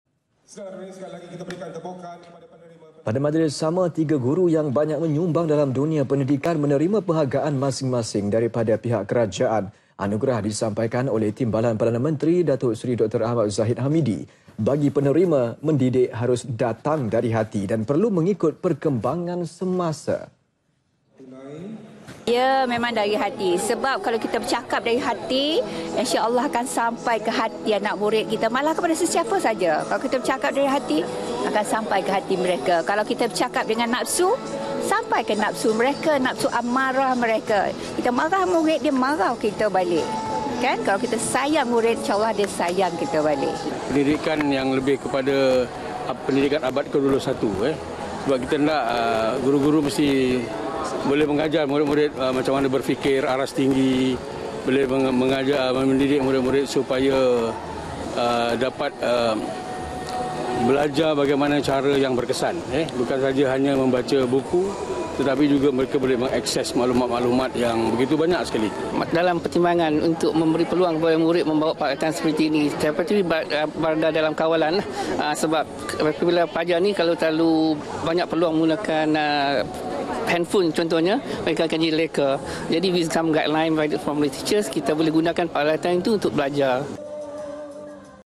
SAMBUTAN HARI GURU – 3 GURU CEMERLANG TERIMA ANUGERAH [16 MEI 2017]
Pada majlis sama, tiga guru yang banyak menyumbang dalam dunia pendidikan menerima penghargaan masing-masing daripada pihak kerajaan. Anugerah disampaikan oleh Timbalan Perdana Menteri, Datuk Seri Dr Ahmad Zahid Hamidi. Bagi penerima, mendidik harus datang dari hati dan perlu mengikut perkembangan semasa.